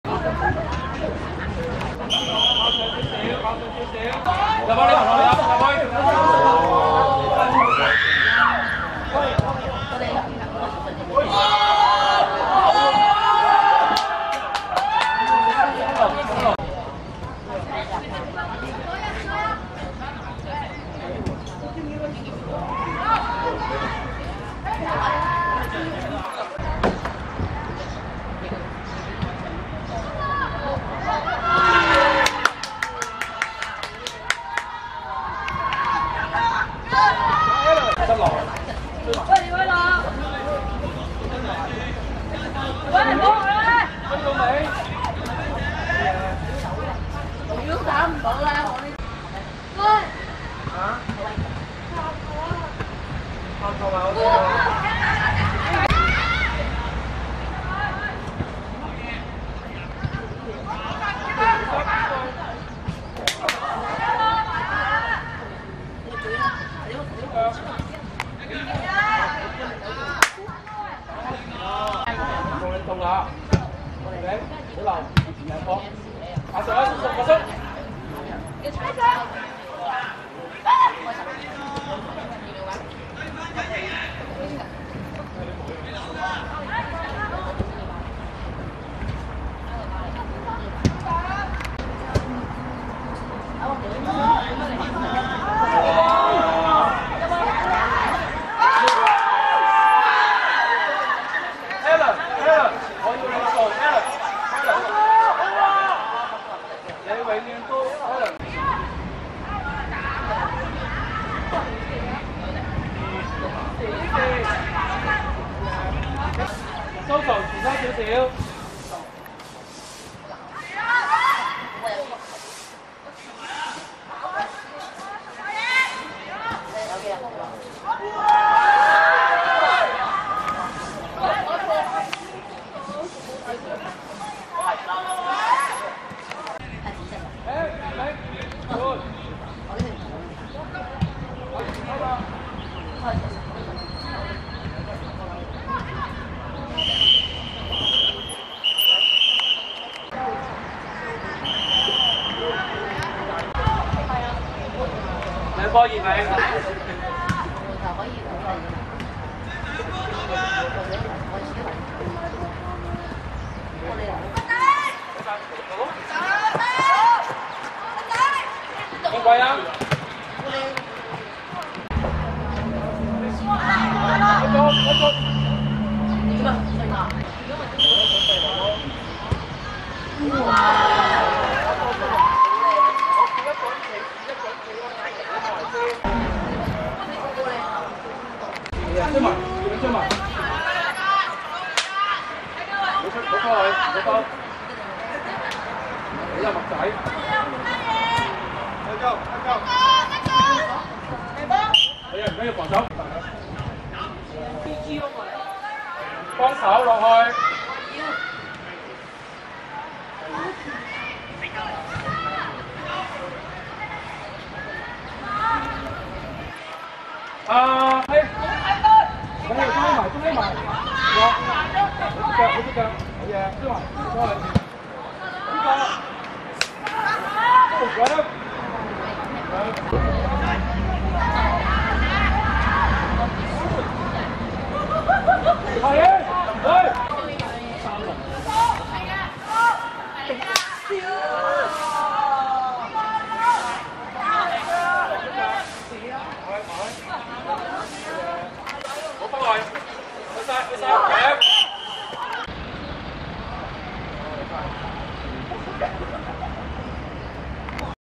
公益社vs天循(2019.4.13.元朗學界足球丙組複賽)精華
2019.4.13.天業路仿真草塲 12:30 公益社(紅) 3 : 2 天循(紫)